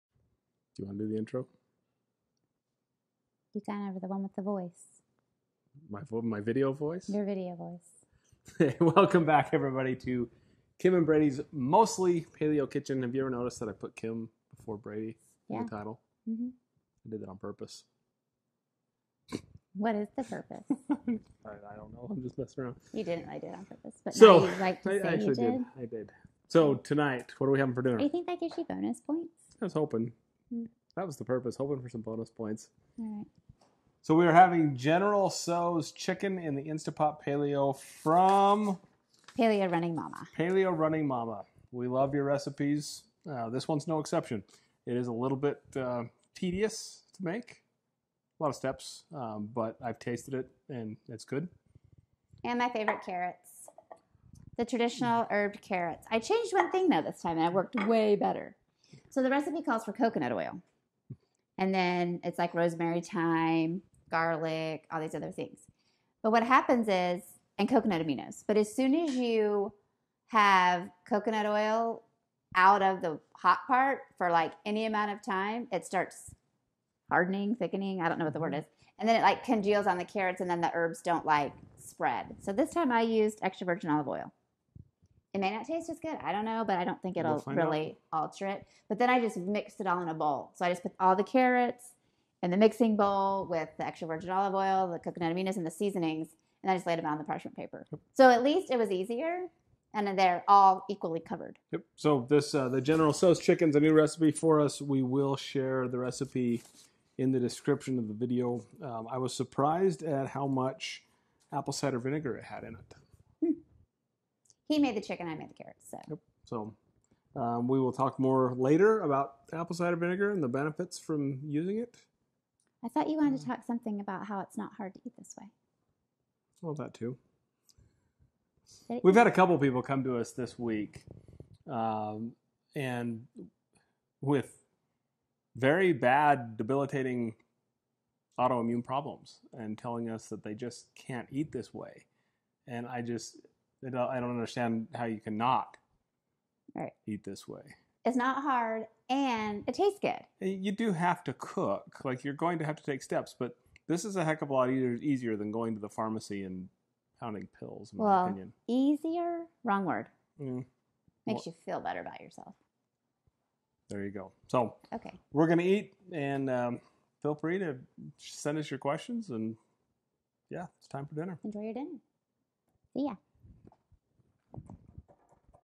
General Tso's Paleo Instant Pot Chicken
2 of our favorite things in this house are Chinese food and the instant pot. BAM. This recipe has both! The kids loved it! We loved it! And this will definitely be in the rotation!